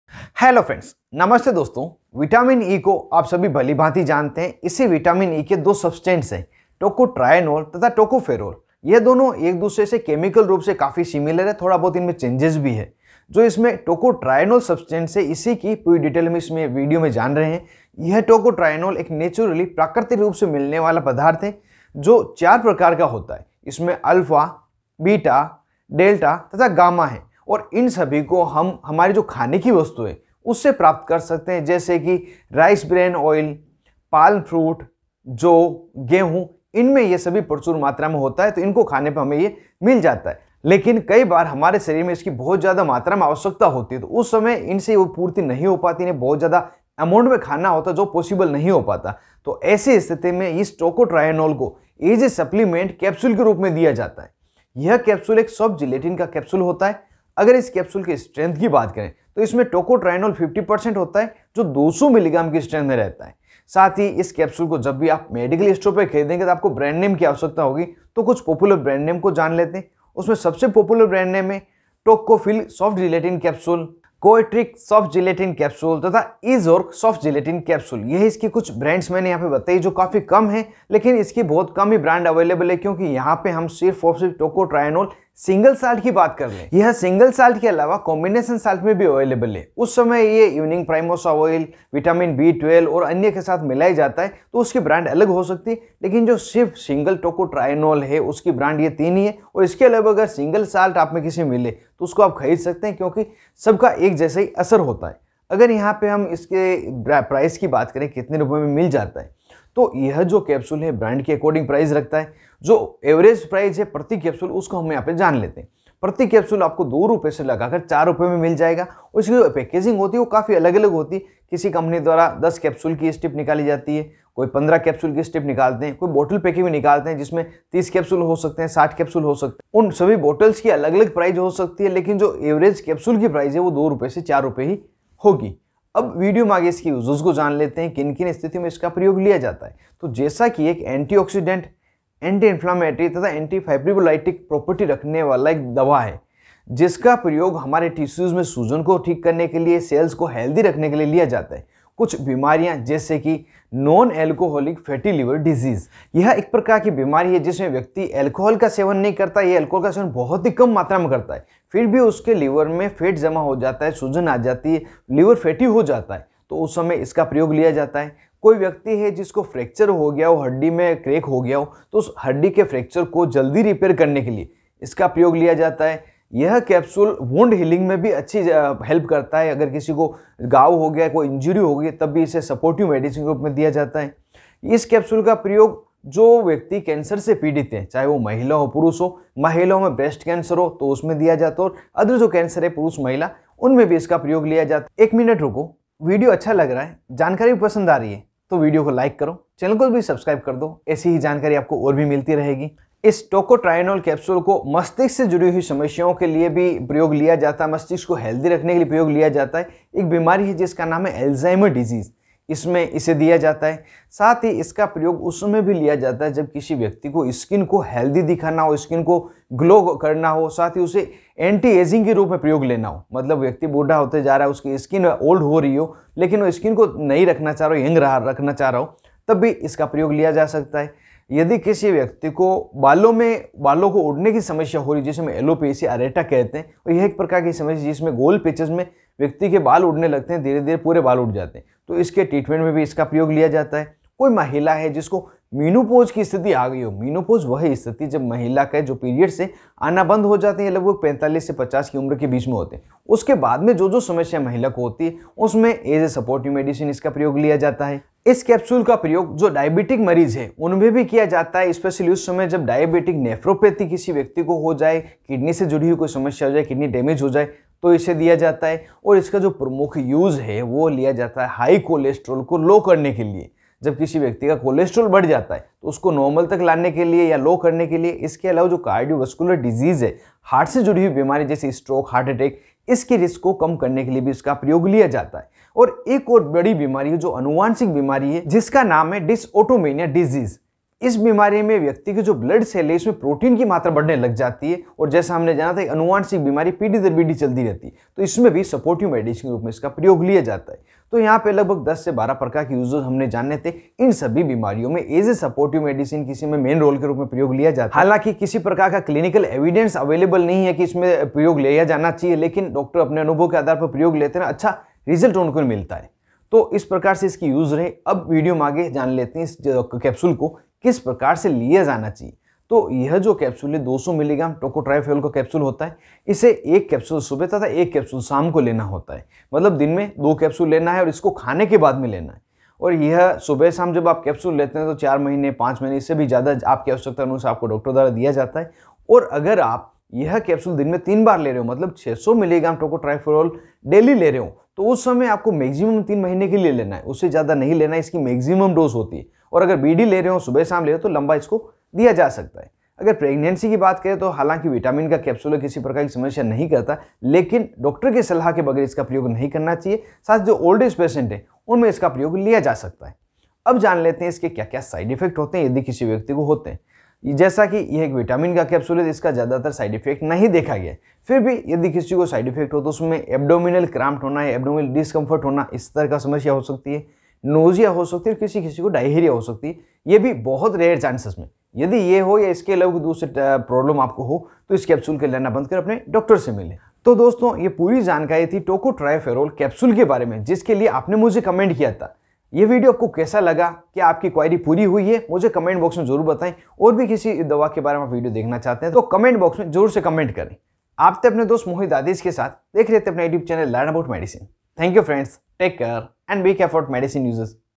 Tocotrienol softgel capsules | Tocofil capsules
Tocotrienols are the substance of vitamin E. available as soft gelatin capsules. Tecotrienol soft gelatin capsules strength is tocotrienol 200 mg.
Tocotrienols softgel capsules brand names are
1. Tocofil softgel capsules
2. Coetrie softgel capsules
This video contains information about Tocotrienol softgel capsules | Tocofil capsules | Tocofil capsules including
1. What is tocotrienols?
2. Tocotrienol soft gelatin capsules strength
3. Tocotrienol capsules brand names
4. Tocofil tablets uses in hindi | tocofil softgel capsules uses in hindi
5. Tocotrienol side effects
6. Tocotrienol dosage